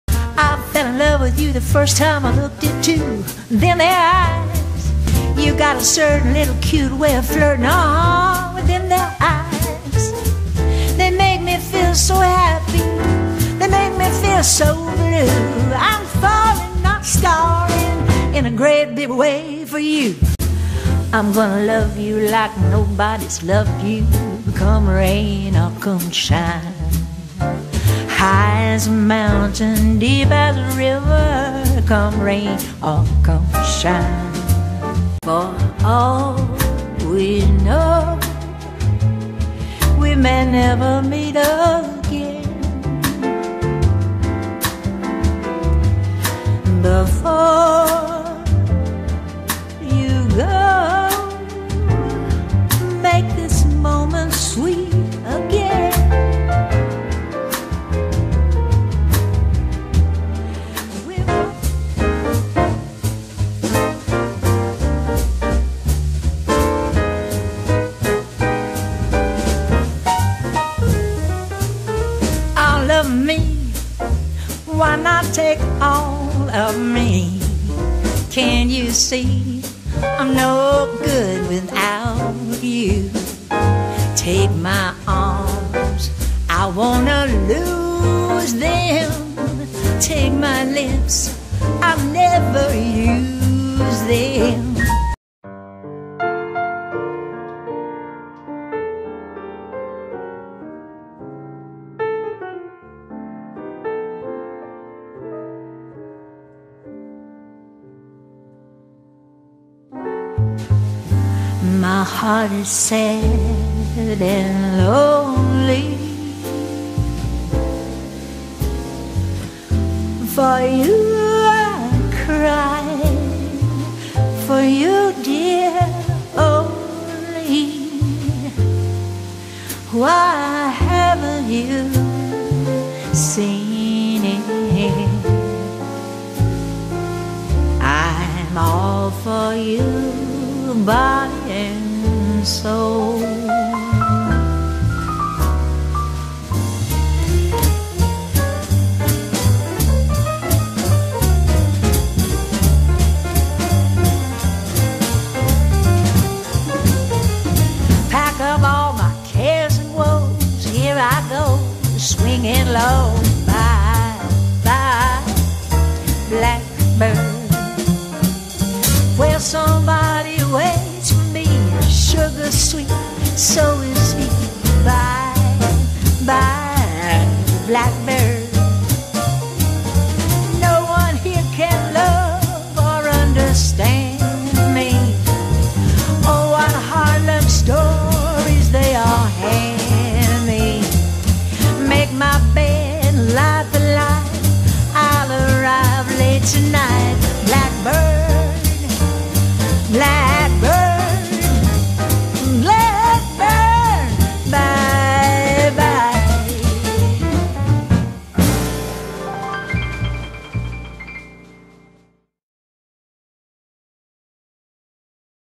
Darlene Shadden paintings music collage
A November 25, 2016 Darlene Shadden sampler; her music and her art.